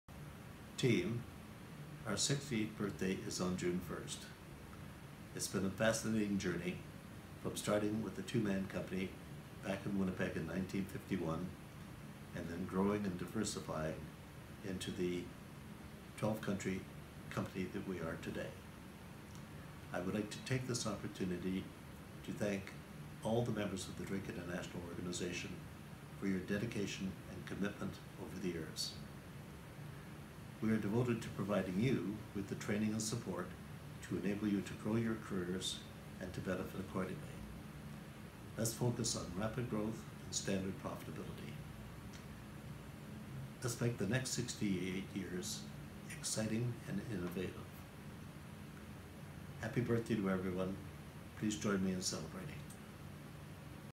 Drake International turns 68
Birthday greetings from our Chairman and Co-founder, Bill Pollock